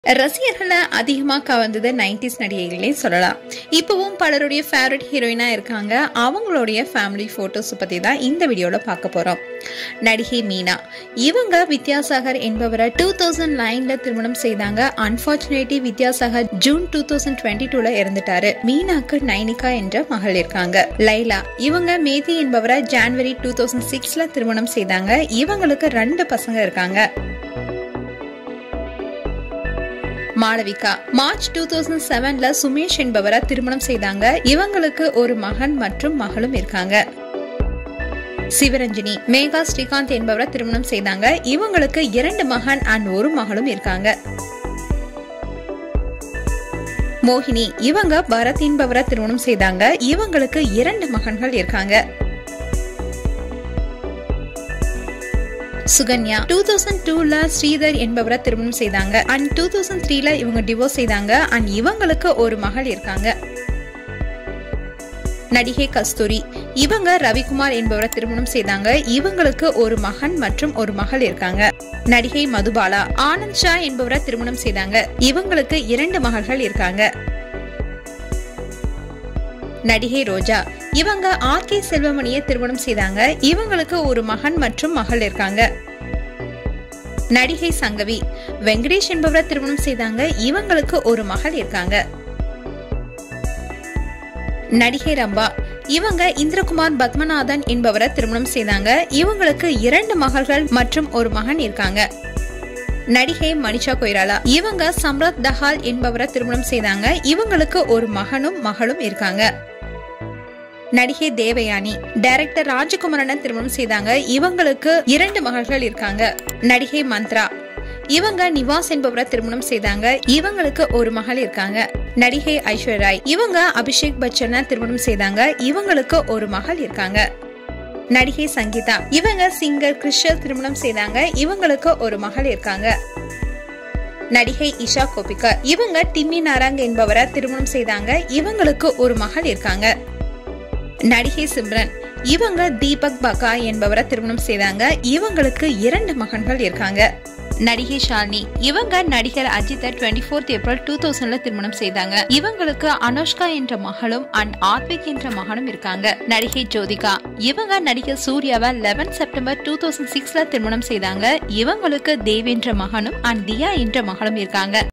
90s Tamil Actresses Beautiful Family Picture's | 90s நடிகைகளின் குடும்பங்கள் | Kollywood Actress
90s Tamil Actresses With Their Family, Kollywood Celebrities With Children, Heroines With Families
90s Actress family photos,  90’s நடிகைகள் குடும்பங்கள்; இப்போ எப்படி இருக்காங்க பாருங்க